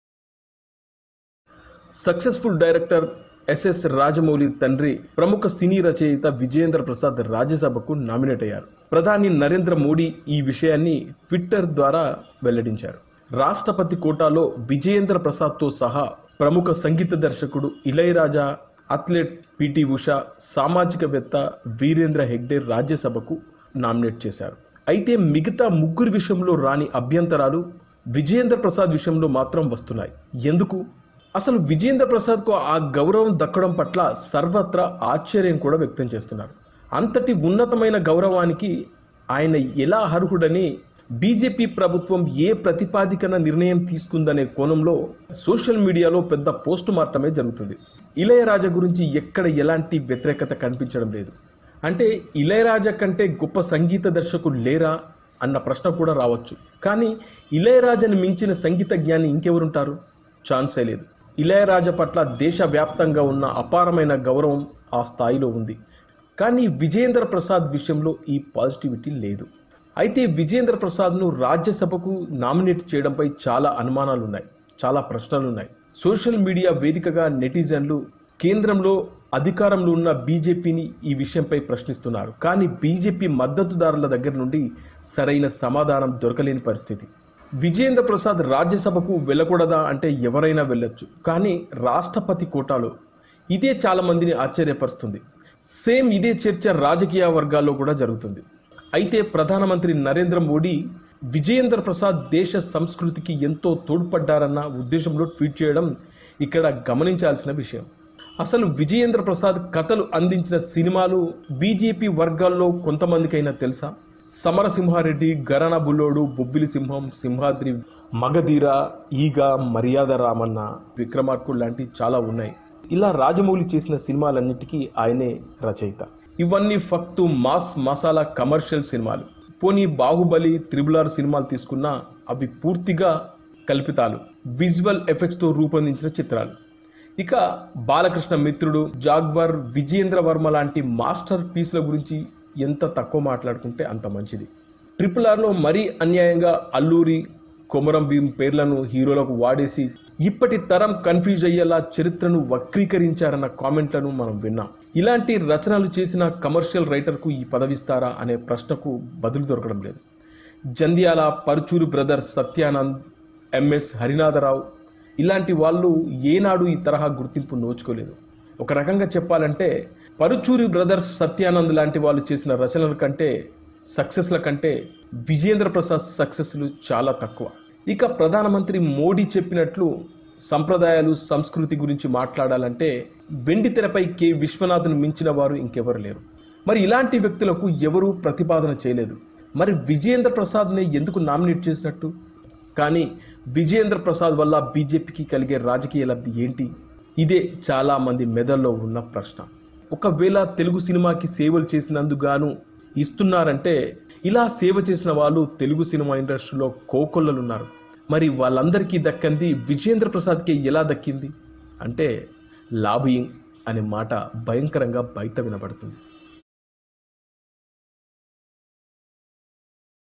Vijayendra prasad : విజయేంద్ర ప్రసాద్ కు రాజ్యసభ ఎలా దక్కింది | Rajiv Erram | Charan tv
రాజమౌళి తండ్రి (Vijayendra prasad) విజయేంద్ర ప్రసాద్ కు రాజ్యసభ ఎలా దక్కింది
Rajiv Erram Comments